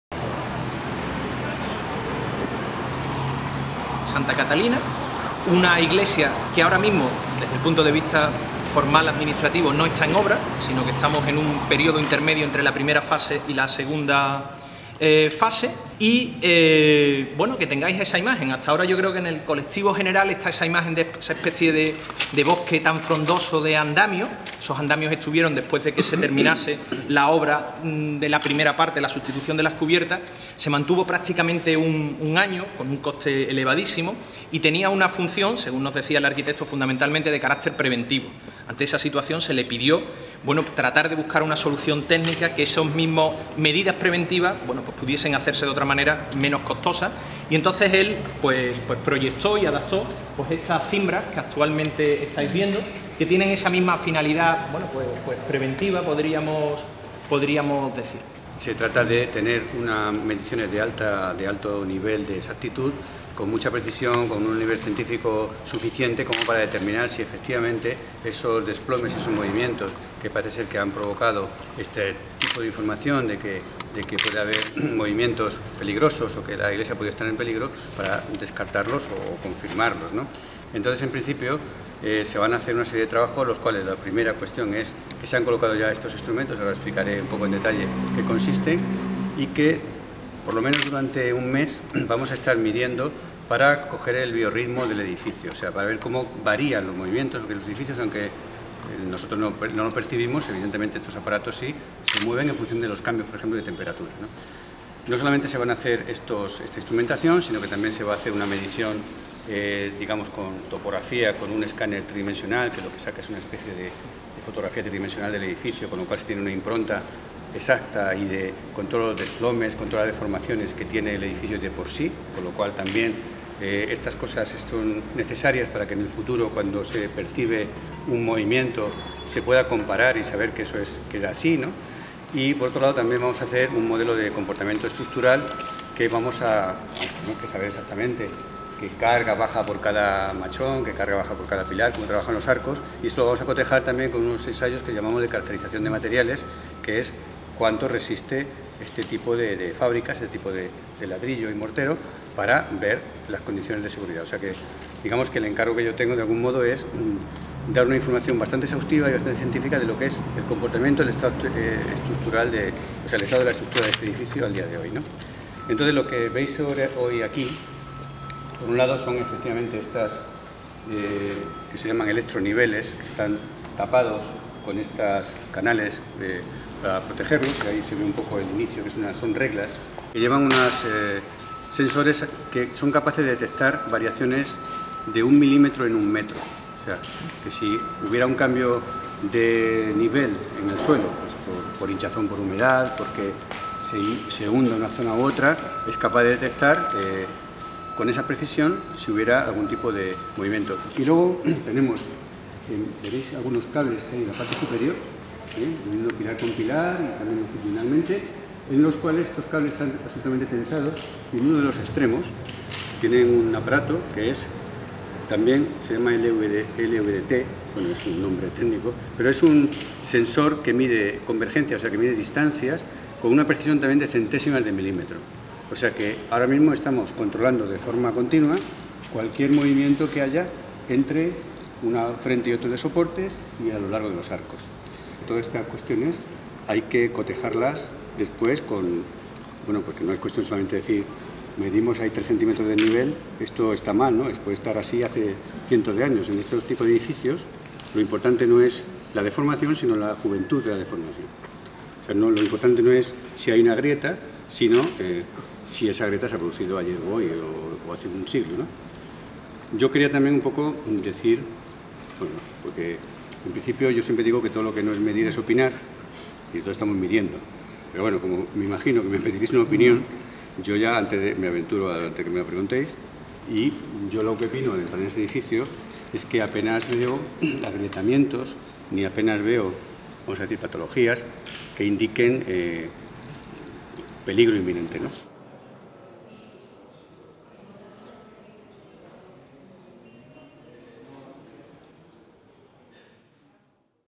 Iglesia de Santa Catalina en Sevilla
Descripción de las intervenciones realizadas en la Iglesia Santa Catalina de Sevilla por temor a debilidades en sus estructuras. Este templo sevillano lleva cerrado al público desde 2004. Video-noticia para el periódico digital Sevilla Directo, emitido el 1 de febrero de 2013.

Imagen: Juanma Moreno